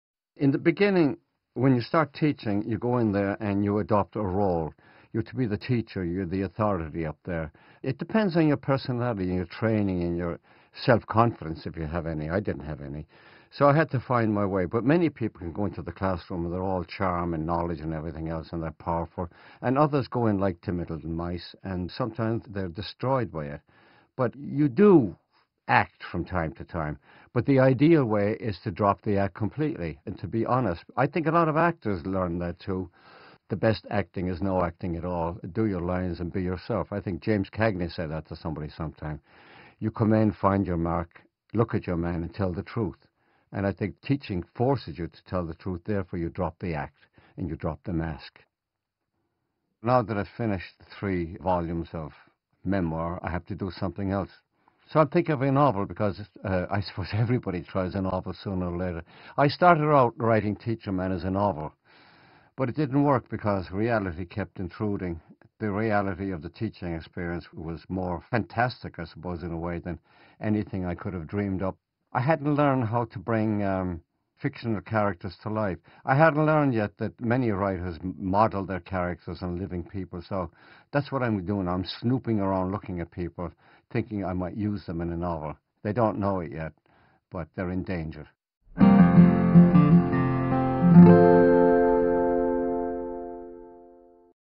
Frank McCourt: Teacher Man
A Pulitzer Prize-winning, #1 "New York Times" bestseller, "Angela's Ashes" is Frank McCourt's masterful memoir of his childhood in Ireland.